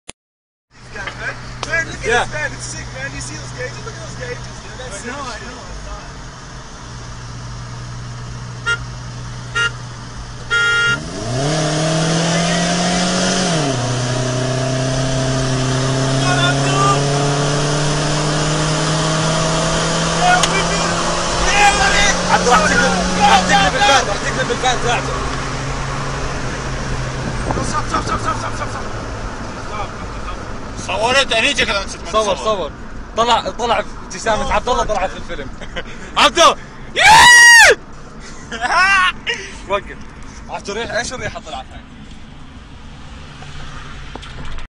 Abdo's minivan(Dodge Grand Caravan) vs. Toyota Previa Twin Turbo
short drag race between two sexy minivans